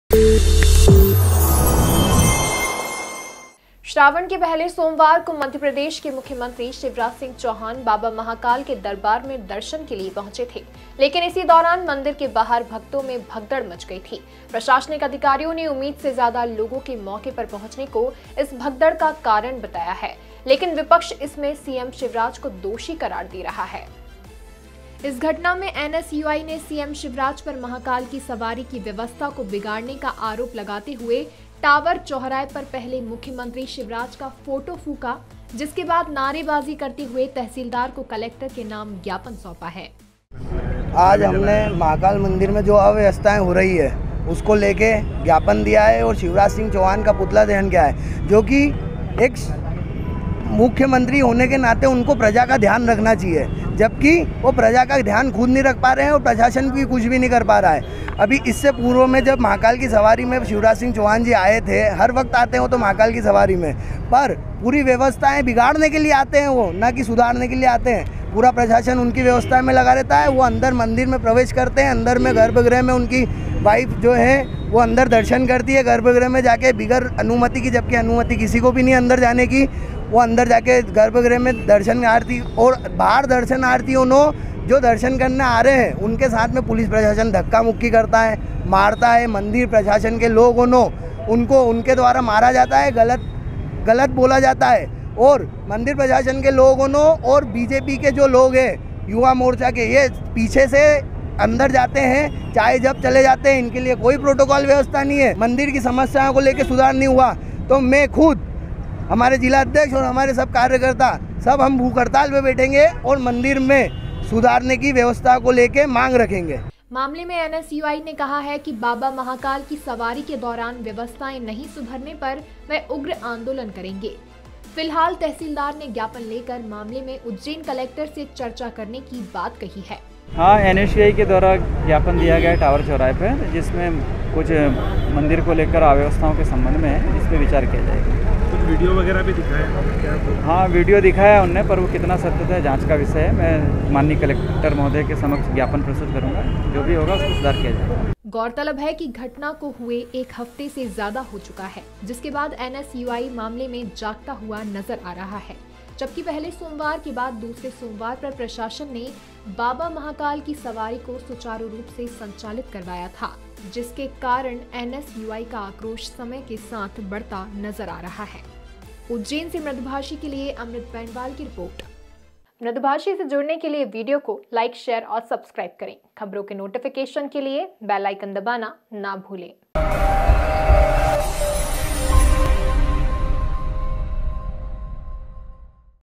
घटना के एक हफ्ते बाद क्यों जागा NSUI? | UJJAIN NEWS | Mradubhashi
श्रावण के पहले सोमवार को मध्य प्रदेश के मुख्यमंत्री शिवराज सिंह चौहान बाबा महाकाल के दरबार में दर्शन के लिए पहुंचे थे। लेकिन इसी दौरान मंदिर के बाहर भक्तों में भगदड़ मच गई थी। प्रशासनिक अधिकारियों ने उम्मीद से ज़्यादा लोगों के मौके पर पहुंचने को इस भगदड़ का कारण बताया है। लेकिन विपक्ष इसमें सीएम शिवराज को दोषी करार दे रहा है।